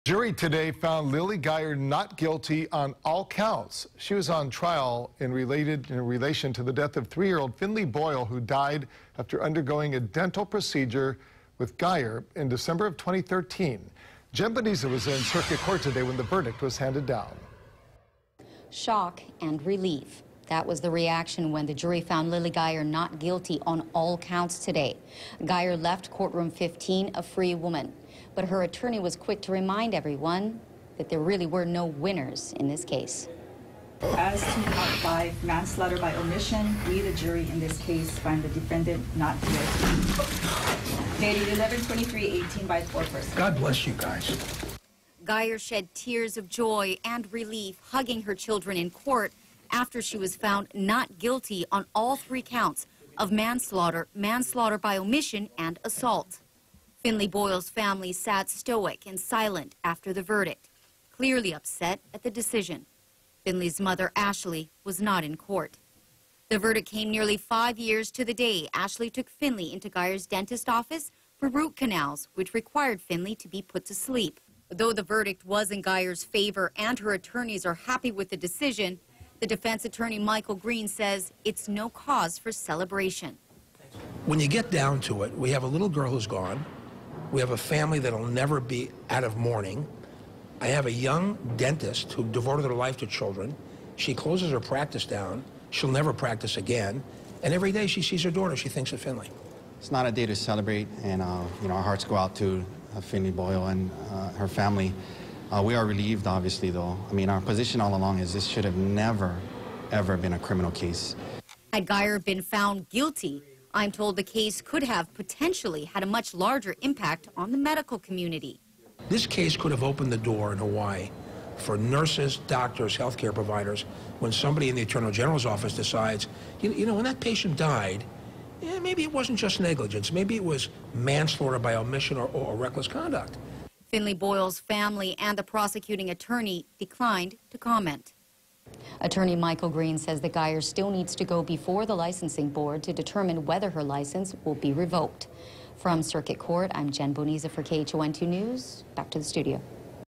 Dentist found not guilty of manslaughter in death of 3-year-old patient
Dentist Lilly Geyer was found not guilty Friday in the death of her 3-year-old patient.